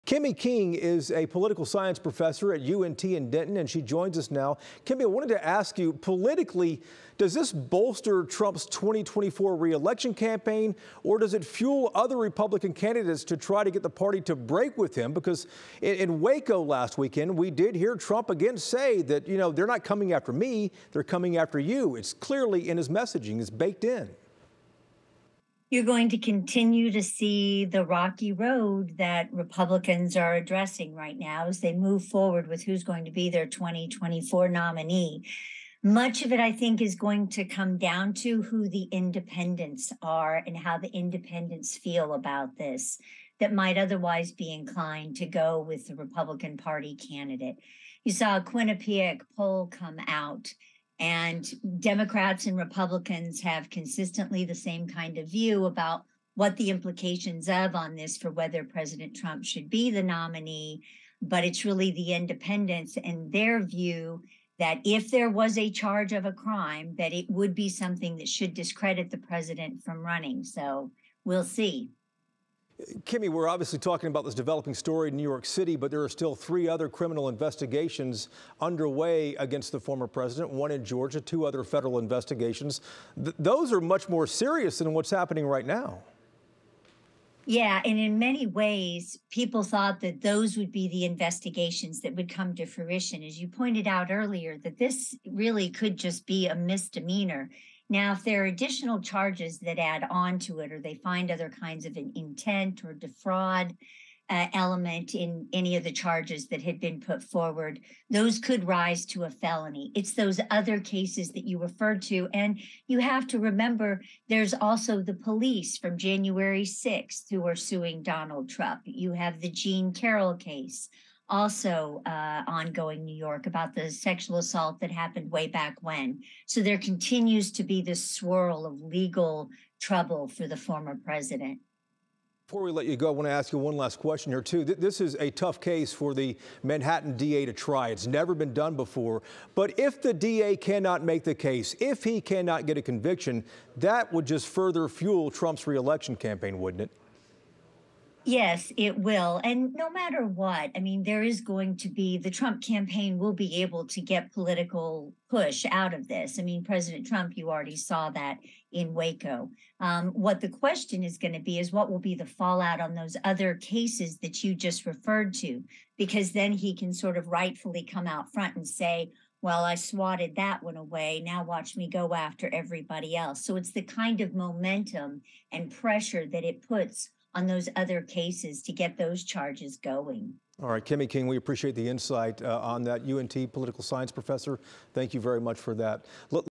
UNT professor weighs in on Trump indictment on hush money case
He is the first former U.S. president to be charged with a crime.